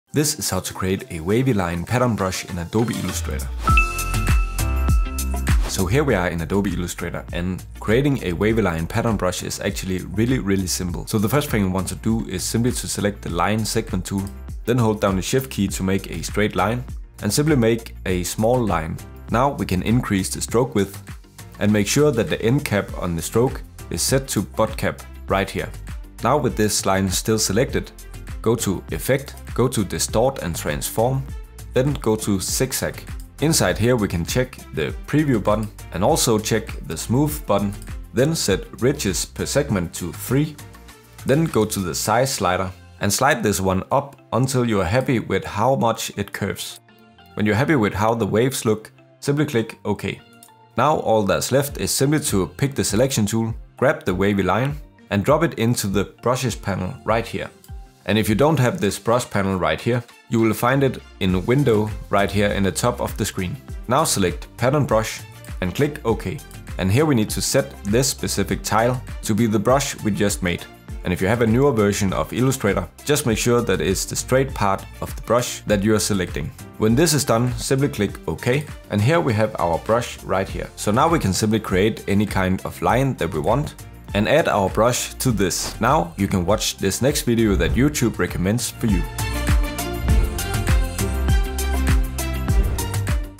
How To Make A Wavy Line Pattern Brush In Adobe Illustrator
Create a wavy pattern brush with a few simple steps.

Here is how to create a wavy line pattern brush that you can apply to all kinds of paths that you create in adobe illustrator. including shapes like cirkles and squares.

📒 Notes 📒

CTRL = Command
ALT = Option